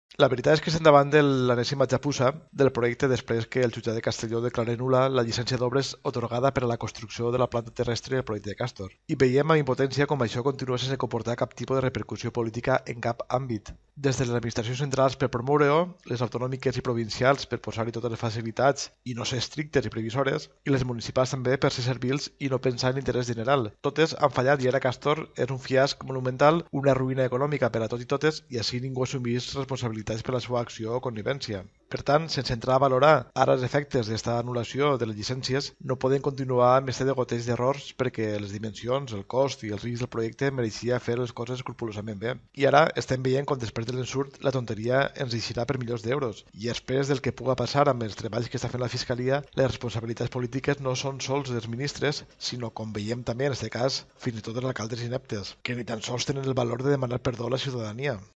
Mulet: “No se puede entender como el desastre del Castor no tiene repercusiones”
El enésimo varapalo al proyecto después de que el juzgado de Castellón declare nula la licencia de obras otorgada a favor de ESCAL UGS para la construcción de la planta terrestre de operaciones del proyecto Castor de almacenamiento de gas al municipio de Vinaroz, continúa sin ocasionar ninguna repercusión política en ningún ámbito, que según el coportavoz de Compromís en las comarcas de Castellón, Carles Mulet, “ desde las administraciones centrales por promoverlo, las autonómicas y provinciales para ponerle todas las facilidades y no ser estrictas y previsoras y las municipales para ser serviles y no pensar en el interés general, todas han fallado y ahora el Castor es un fiasco monumental, una ruina económica para todos y todas, y aquí, nadie asume responsabilidades por su acción o connivencia”

Según Mulet, la declaración como nulo por parte del contencioso-administrativo núm. 2 de la licencia de obras otorgada por el Ayuntamiento de Vinaroz en 2009, para la construcción de la planta terrestre de operaciones, es la enésimo episodio del despropósito.

Ahora, un sentencia declara nula la licencia de obras otorgada por el Ayuntamiento de Vinaroz a favor de la empresa ESCAL UGS para la construcción de la planta terrestre de operaciones del proyecte Castor de almacenamiento de gas por no es compatible urbanísticamente por exceso de edificabilidad del proyecto de ejecución; y también porqué , previamente a la concesión no se hubiera emitido la preceptiva declaración de interés comunitario, todo por la precipitación, puesto que el mismo día que se presentó la solicitud de la empresa promotora los técnicos municipales informaban un proyecto de muchos volúmenes de información.

Según Mulet “ sin entrar a valorar ahora los efectos de esta anulación de licencias, no podemos continuar con este goteo de errores, porque las dimensiones, coste y riesgo del proyecto merecía hacer las cosas escrupulosamente bien, y ahora estamos viendo como después del susto, la tontería nos sale por millones de euros, y a esperas de lo que pueda pasar con los trabajos que está haciendo la Fiscalía, las responsabilidades políticas no son sólo de los ministros, sino, como vemos en este caso, hasta y todos de alcaldes ineptos, que ni siquiera tienen el valor de pedir perdón a la ciudadanía”